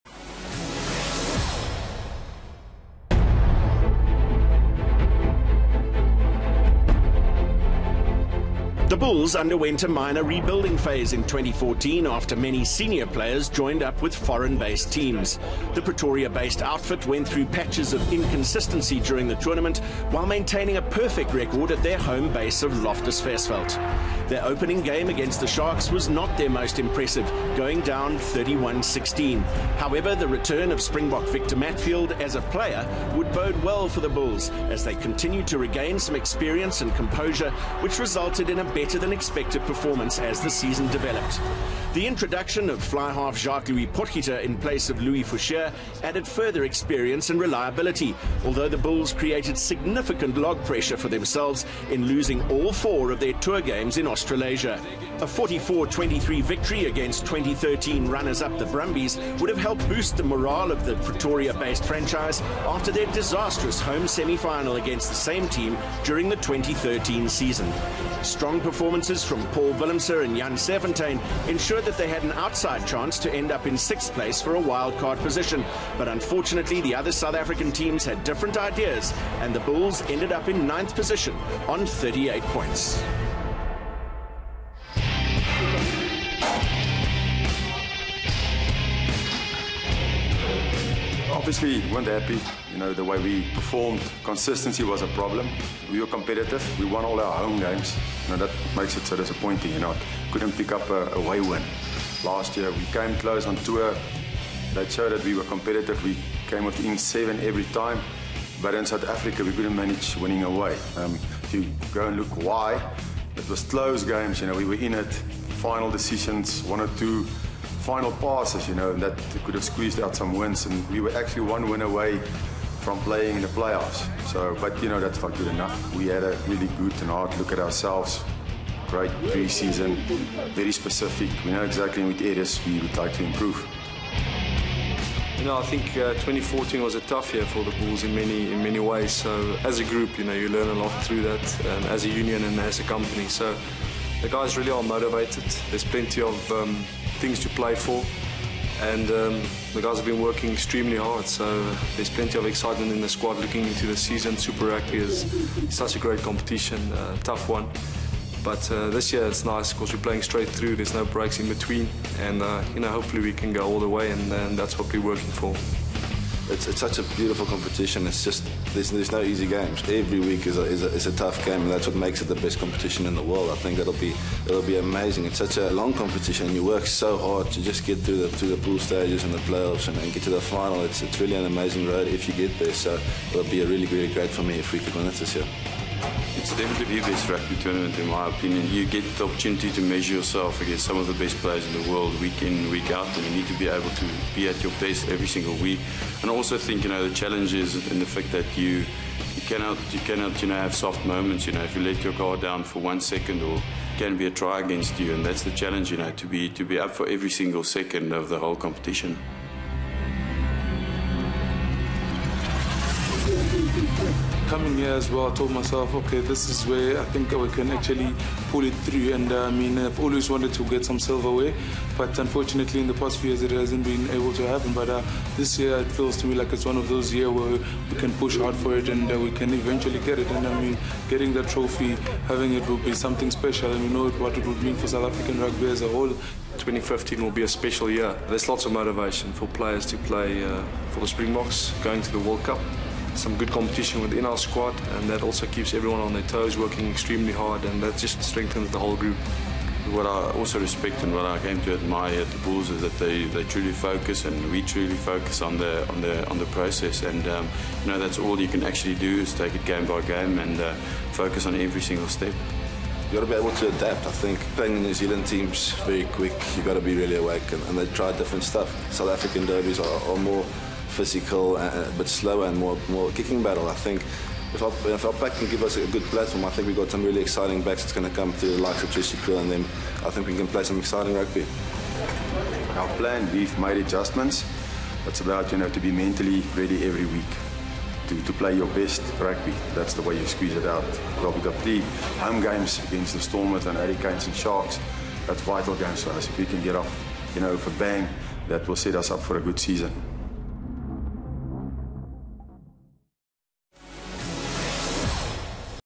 We go behind the scenes with the Vodacom Bulls
A behind the scenes look at the Vodacom Bulls training session ahead of their Vodacom Super 15 season opener against the DHL Stormers this weekend.